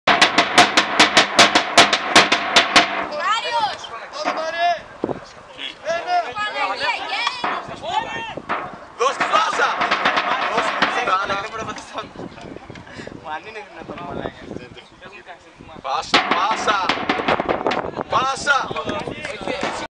Μαλιακός-Πελασγιακός 6-2, Φάση 8η, Φιλικός Αγώνας, Σαβ 16 Σεπ 2017 DSCN6373
Φιλικός αγώνας μεταξύ του Μαλιακού και της ομάδας μας, που διεξήχθη στο στάδιο Στυλίδας, το απόγευμα του Σαββάτου 16 Σεπ 2017, με σκόρ 0-0 στο ημίχρονο και 6-2 τελικό. Τα γκόλ για την ομάδα μας οι Μάνος Χάρης και Ράπτης Αντώνης Κ.